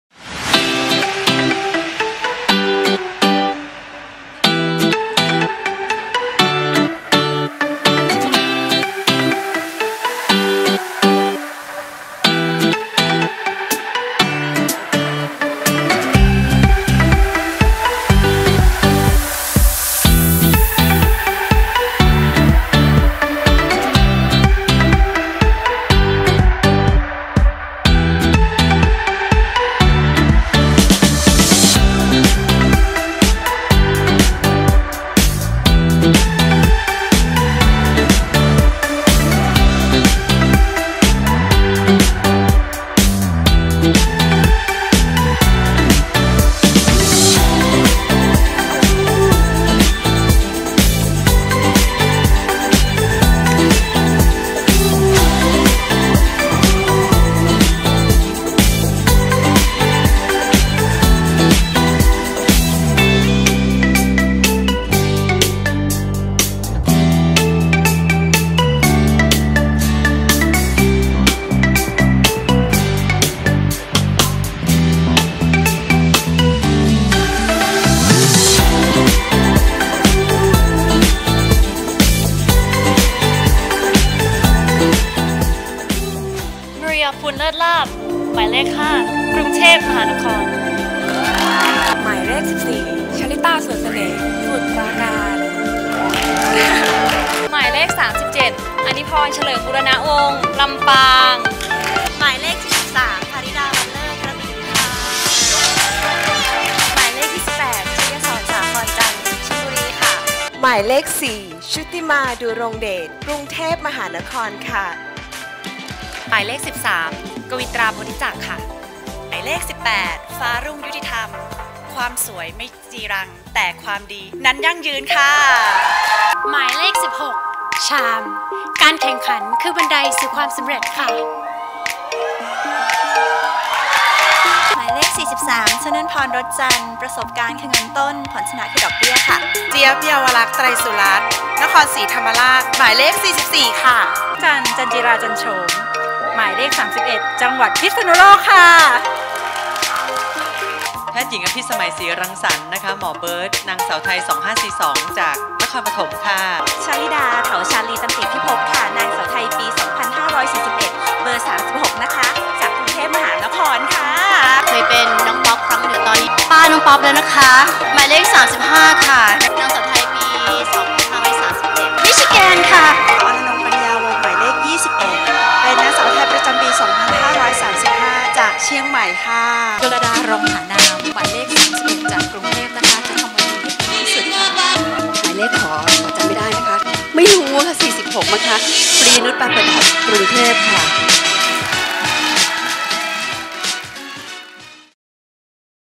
บรรยากาศวันรับสมัคร Miss Universe Thailand 2018
บรรยากาศการรับสมัคร Miss Universe Thailand 2018 และมาหวนคิดถึงการแนะนำตัวของคณะกรรมการรุ่นพี่นางงามระดับตำนาน  ขอบอกว่าแต่ละคนไม่มีใครยอมใครเลยค่ะ